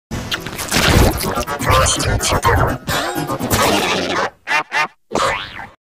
I Made Seal Major On Kinemaster
How To Make Seal Major:

Rotate/Mirroring
Mirroring: Flip

Apply Scene Riplica, and edit:
Ignore "bottom": 1
Top: 3
Bottom: 2
Space: 0
Mirror: 0

Apply Fisheye/Fisheye 2, and edit:
Stregth: 25

Apply: Color Changer, and edit:
Color Offset: 360
Hue: -180
Saturation: 0
Lightness: 0
Contrast: 0
Vibrance: 0
Shadow: 0
Highlight: 0
Gain: 0
Lift: 0
Gamma: 0
Temperature: 0

Pitches:
Voice Changer: Man + Pitch: -1
Ptich: 5
Voice Changer: Helium + Pitch: -3